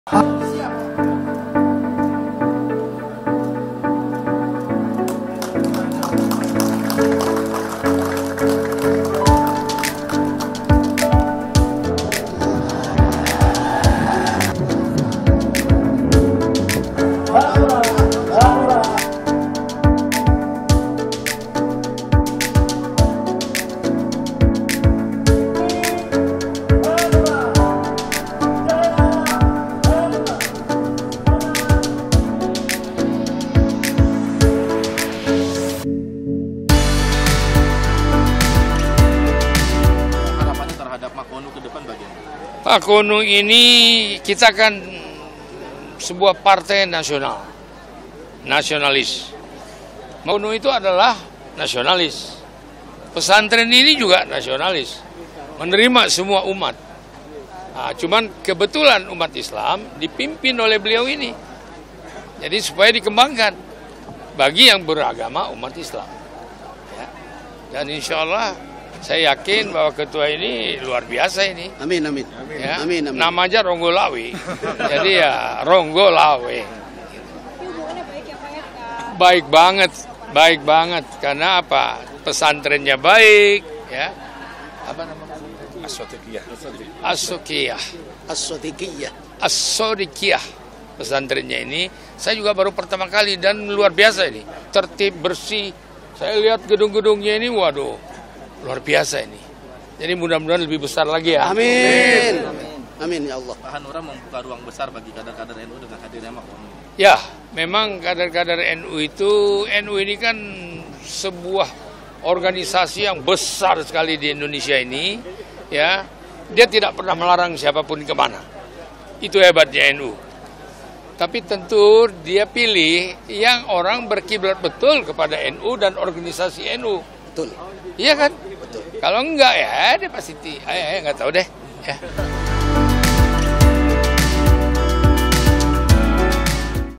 Lebarkan Sayap Organisasi, Ketum Hanura OSO Lantik Majelis Qolbu Nurani
SEMARANG, KOMPAS.TV - Jelang pemilu 2024 mendatang, berbagai persiapan dilakukan Partai Hanura. Salah satunya melebarkan sayap organisasi partai dengan melantik Majelis Qolbu Nurani (Maqonu).

Pelantikan ini digelar di Pondok Pesantren Asshodiqiyah, Semarang, Jawa Tengah pada 29 Mei 2023 dan turut dihadiri para kyai, ulama hingga Ketua Umum Partai Hanura Oesman Sapta Odang (OSO).

Di acara pelantikan, Oesman menjelaskan sejarah bangsa tak lepas dari besarnya peran kyai dan ulama. Oesman juga membuka ruang untuk para kader-kader NU dengan hadirnya Maqonu.

"NU ini kan sebuah organisasi yang besar sekali di Indonesia ini. Dia tidak pernah melarang siapapun kemana. tapi tentu dia pilih orang yang berkiblat betul," ujar Oesman.

Di situasi khususnya menjelang pemilu 2024, Oesman juga meminta restu menyatukan sinergi bersama demi mewujudkan Indonesia Makmur, salah satunya dengan memajukan daerah-daerah di Indonesia.

Acara pelantikan ini berlangsung cukup meriah, juga turut disaksikan oleh para santri Pondok Pesantren Asshodiqiyah.

Baca Juga Hadiri Rakernas IKA PMII, Ketum Hanura OSO Harap PMII Semakin Maju di